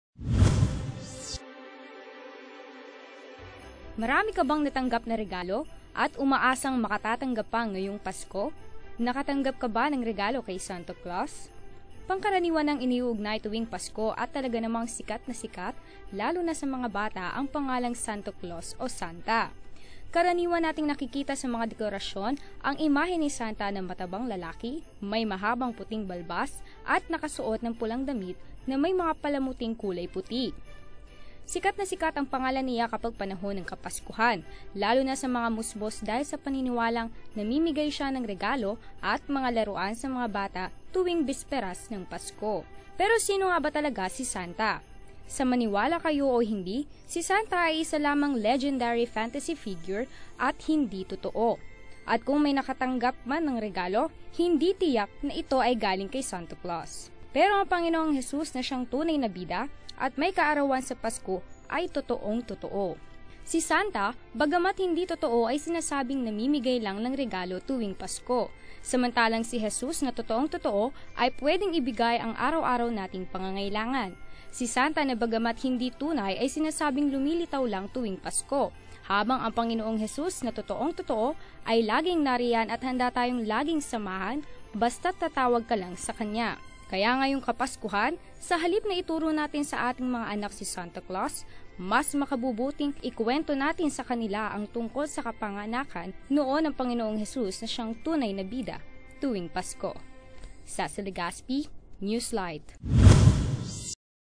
SINO BA SI SANTA? | December 24, 2018
PANOORIN:

Sino ba bida sa mga anak ninyo kapat ganitong kapaskuhan? Aasa ba sila mamaya kay santa claus na bibigyan sila ng regalo? Naku eh makabubuting pakinggan n’yo muna ang pag-uulat na ito.